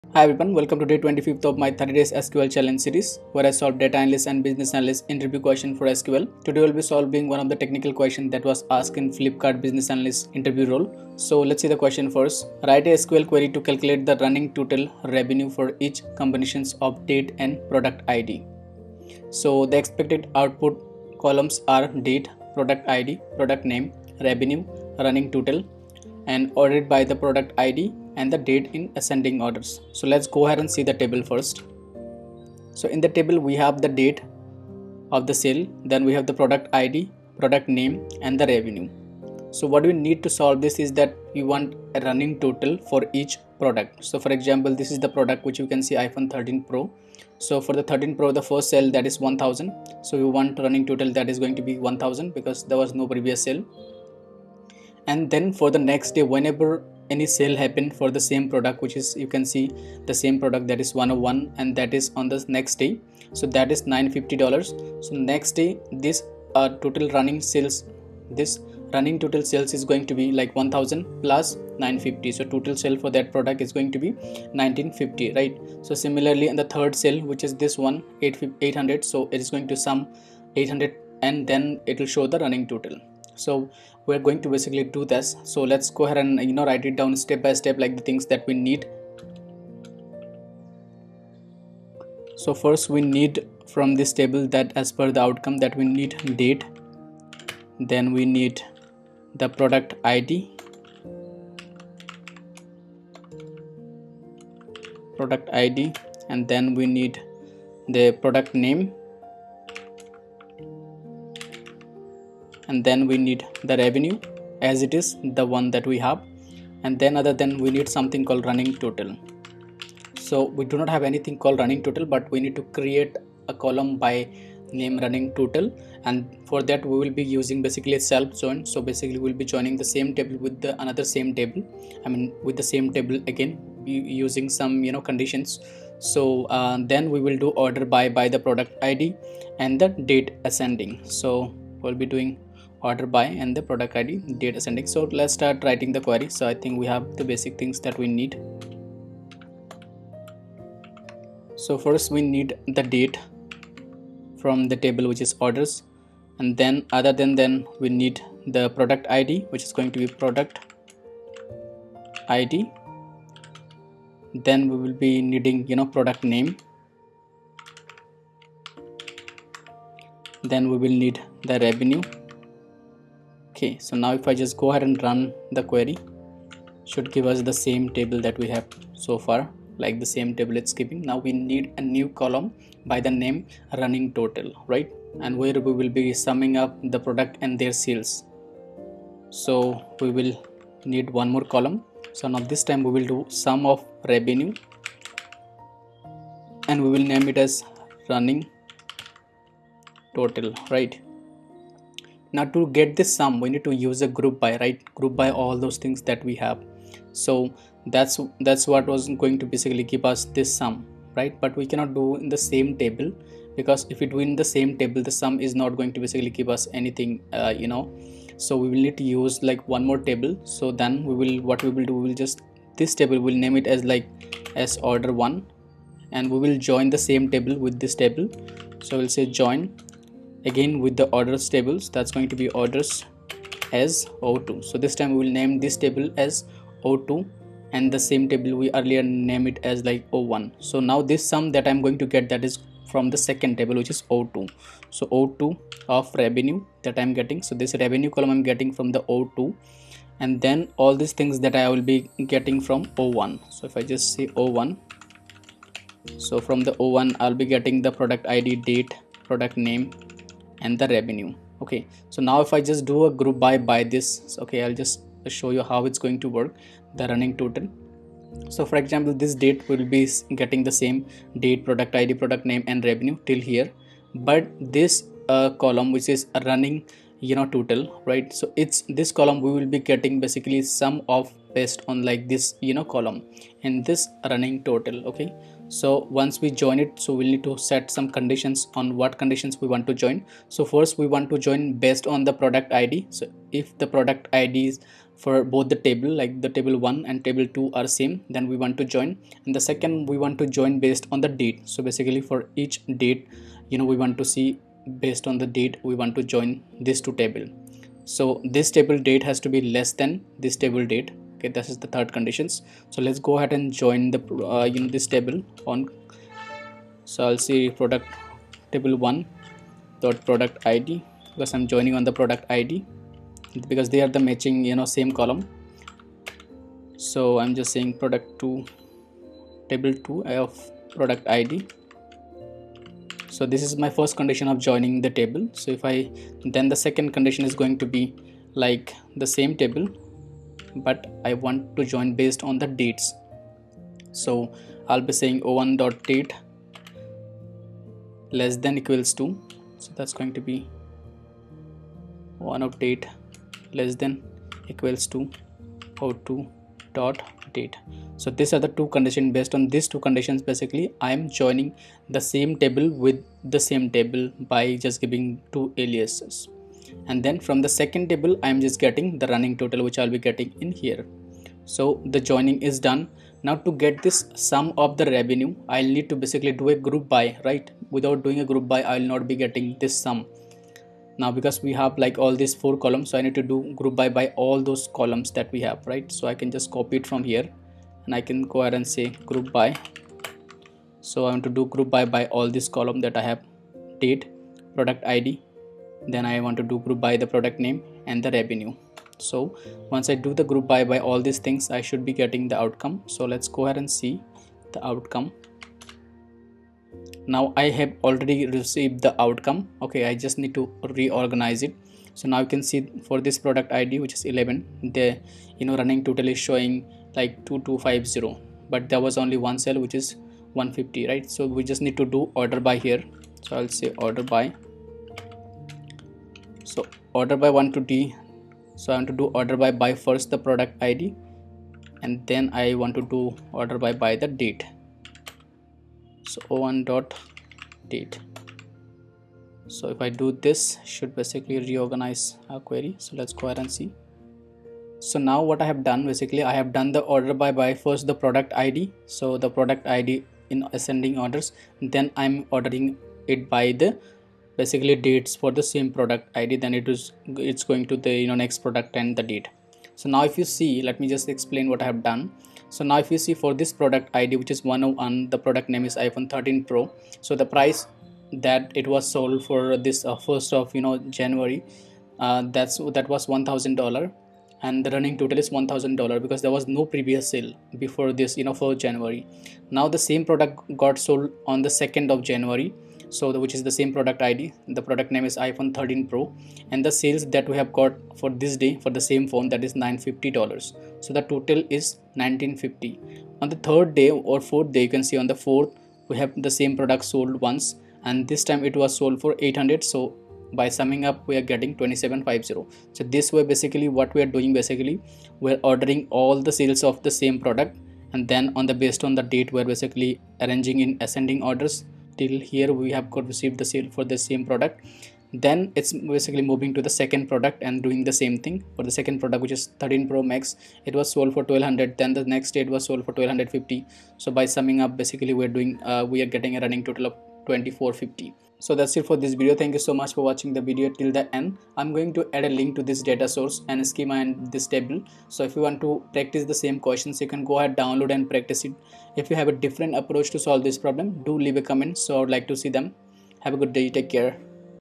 Top SQL Interview Questions and Answers | 30 Days SQL Challenge |SQL Tutorials |Flipkart - #25/30
Get all my latest SQL project!

Project 8: (Real Business Problems)

Ready to CRUSH your Data Analyst or Business Analyst interview? Join me on this 30-DAY SQL INTERVIEW CHALLENGE!

Understanding the problem statement: We'll break down the question clearly and identify key requirements.
Building the optimal SQL query: Step-by-step, we'll craft an efficient and accurate solution using JOINs, aggregations, and filters.
Explaining your thought process: Learn how to articulate your problem-solving approach and showcase your SQL expertise.
BONUS TIPS & TRICKS: Gain valuable insights for acing your SQL interview with confidence.

Music cc:  YouTube-Channel Liborio Conti